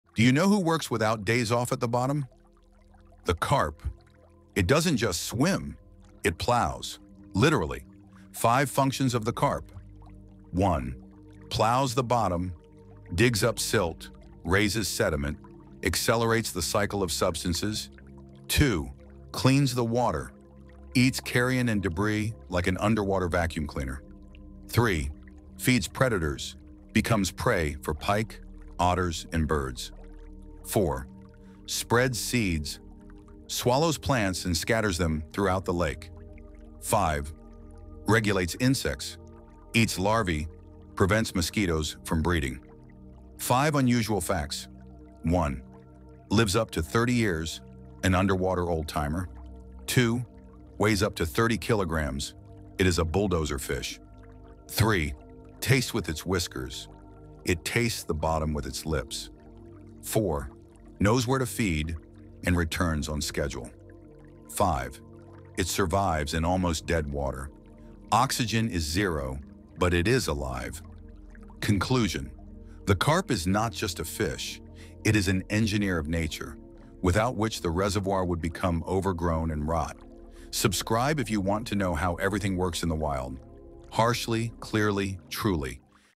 Wild Carp — Nature’s Secrets in 90 Seconds
In just a minute, learn why the wild carp surprises scientists and how it’s vital for ecosystems.